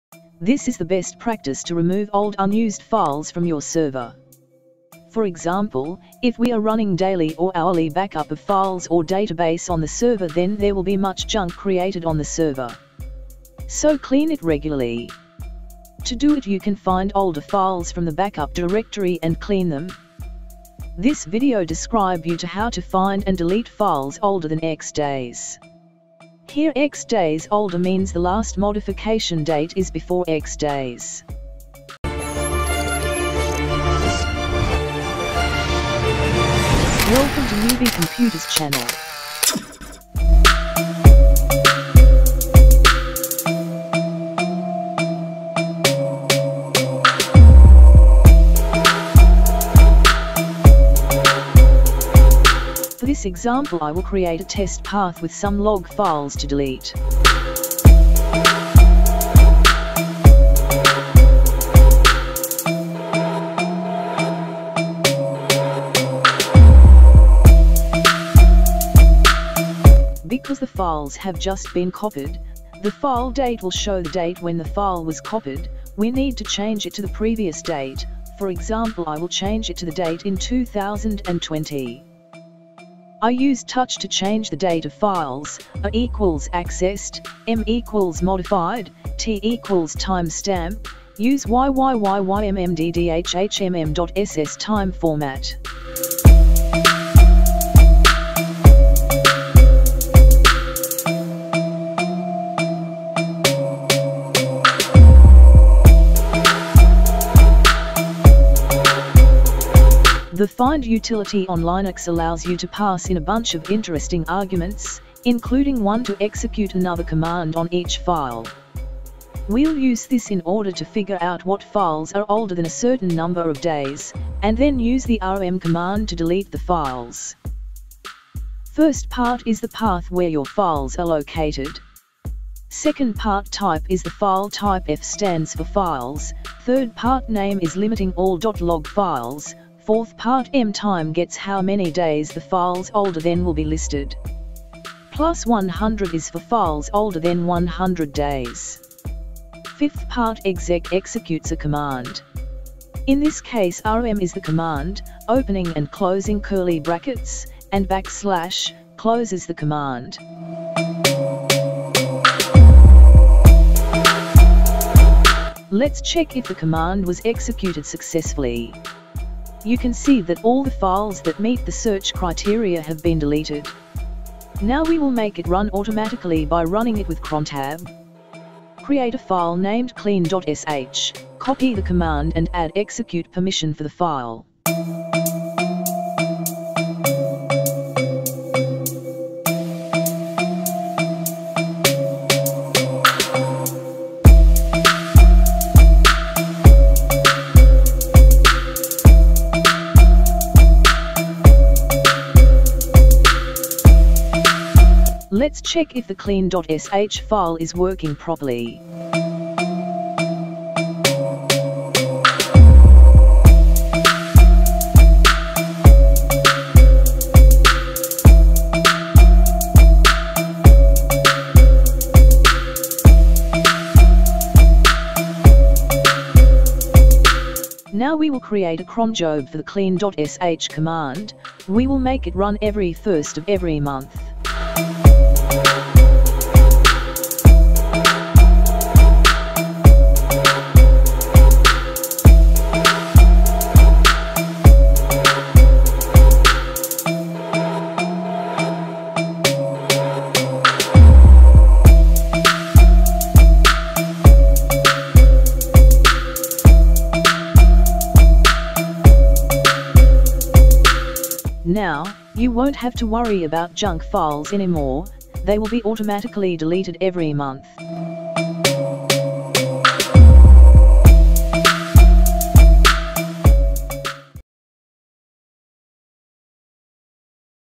how to automatically delete/remove files older than x days in linux - crontab/cronjob-easy tutorial
This is the best practice to remove old unused files from your server. For example, if we are running daily or hourly backup of files or database on the server then there will be much junk created on the server. So clean it regularly. To do it you can find older files from the backup directory and clean them. This video describe you to how to find and delete files older than X days. Here X days older means the last modification date is before X days.